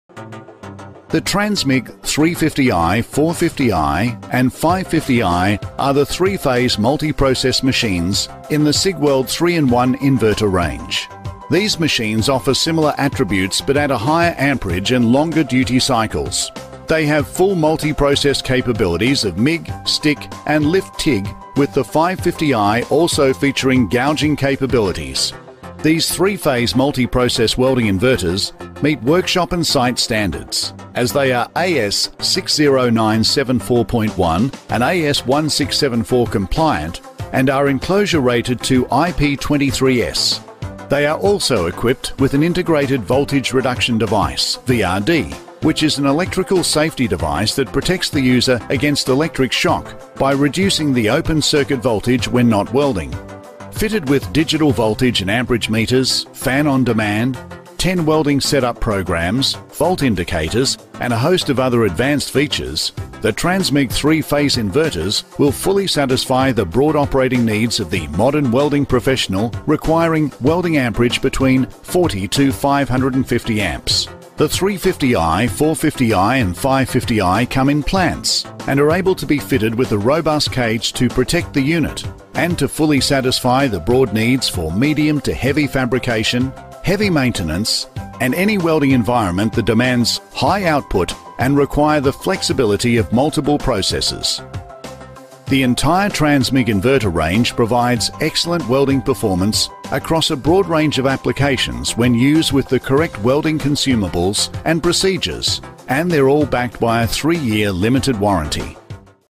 CIGWELD 3 Phase Multi-Process Inverters
CIGWELD's 3 Phase Multi-Process Inverter range delivers 3in1 MIG, Stick and TIG capability from one integrated system. The 350i, 450i and 550i inverter welding power sources are fully featured and delivery outstanding arc characteristics; combine a compact, efficient, portable design with a full range of packaged accessories and you have the ultimate fully featured professional range for construction, mining, shipbuilding and other industrial applications.